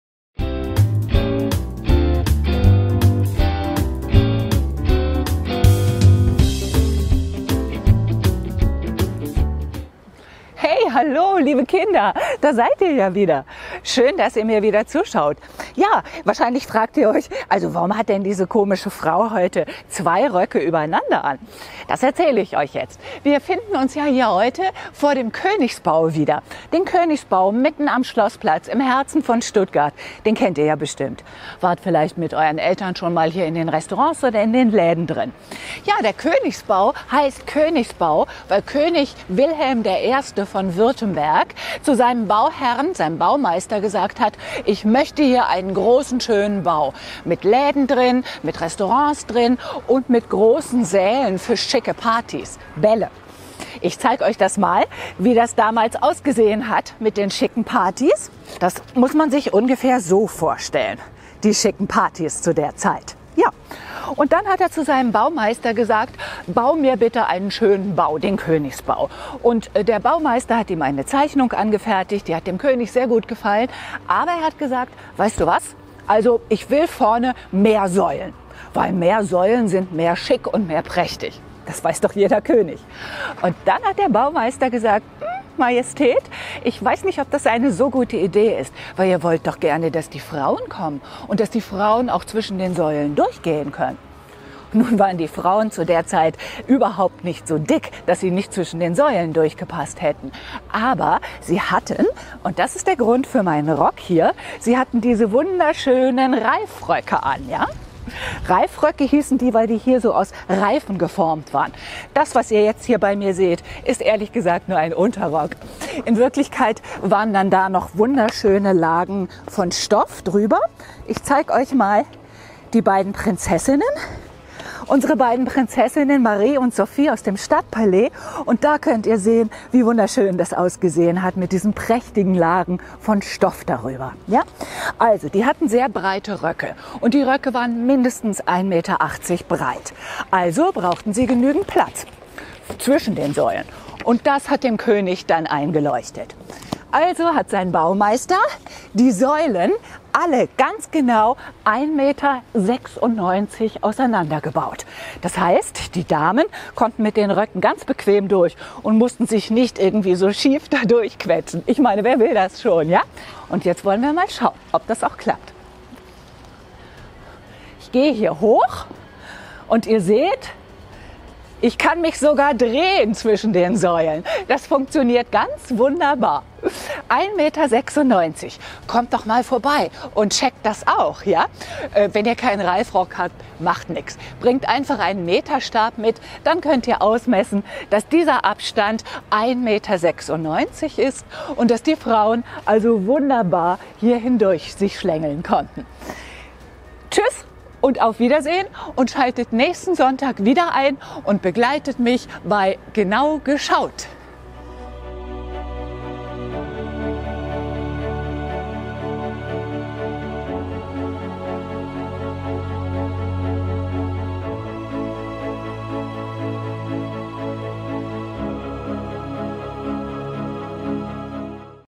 Genau geschaut // Die Säulen des Königsbaus
Was ist so besonders an den vielen Säulen am Königsbaus? Warum stehen die so weit auseinander? Dieses Geheimnis wird am Sonntag von Barbara von Münchhausen gelüftet, es wieder heißt - Genau geschaut.